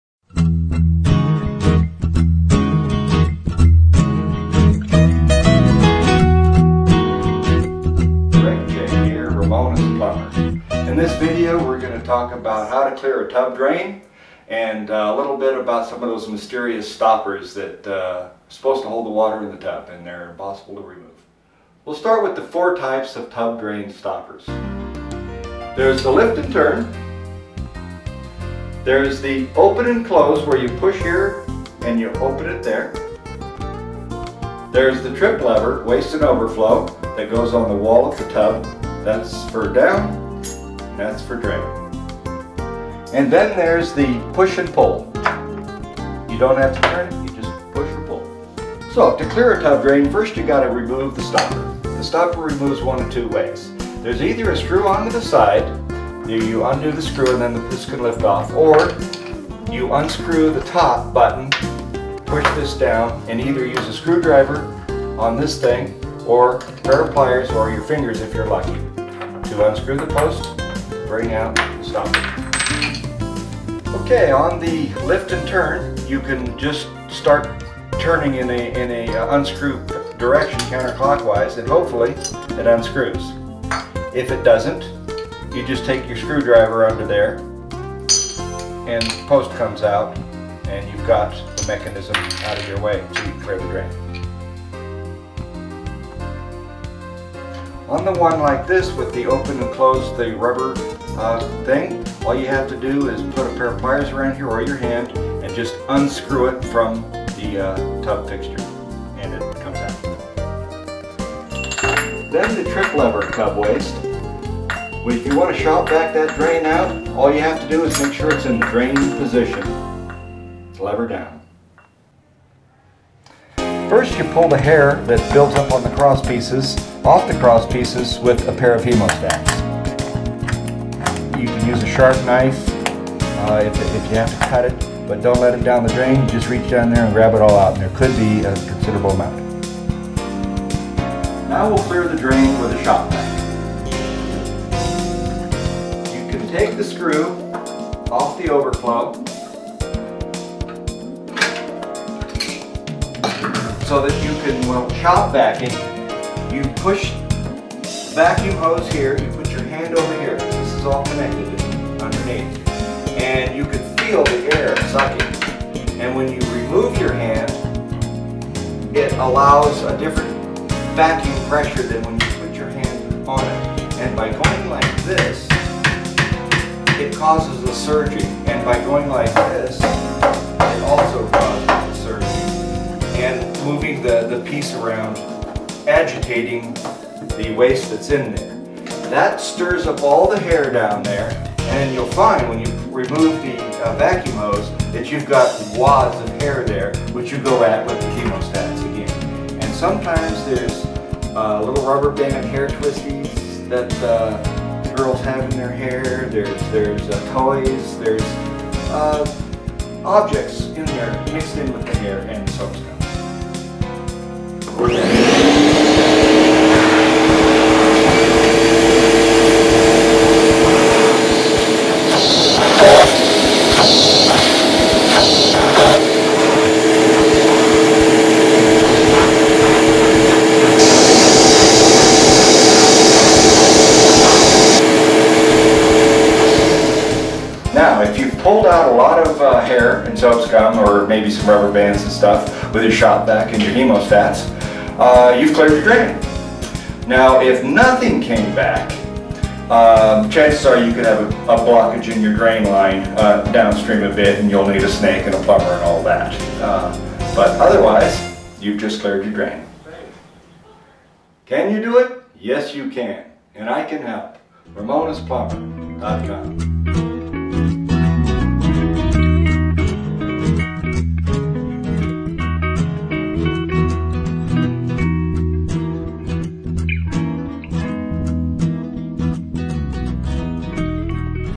How to Clear a Bathtub Drain with a Shop Vac
Greg Chick, Ramona's Plumber shows how to clear a bathtub drain using a shop vac.
Four common types of shower stoppers are discussed and how to get them out of the way for the cleaning operation.

PLEASE COMMENT ON AND RATE THIS VIDEO
Ramona's Plumber, Ramona, California